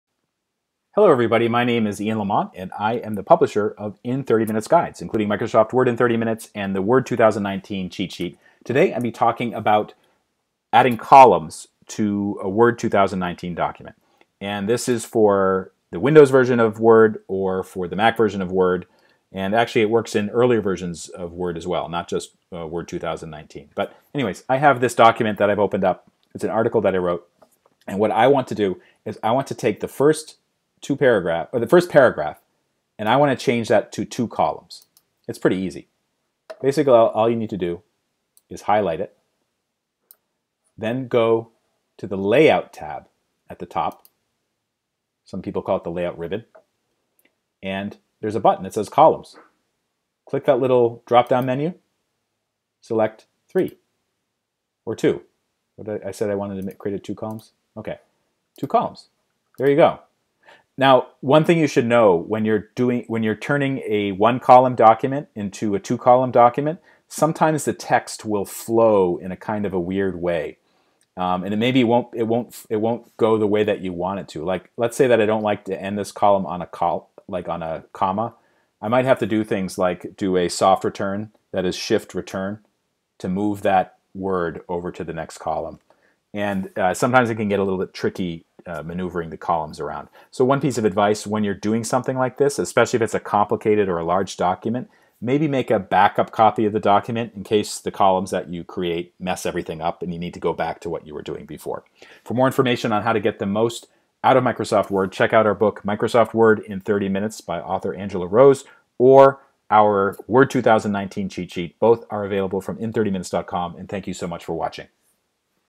Microsoft Word: Make document text 2 columns
Learn how to make document text 2 columns, 3 columns, or more in a Word 2019 document (Word 2019 for Windows or Word 2019 for macOS). Also shows a tip for shifting column text from one column to another. Narrator Ian Lamont is the publisher of IN 30 MINUTES guides, including MICROSOFT WORD IN 30 MINUTES and the WORD 2019 REFERENCE AND CHEAT SHEET.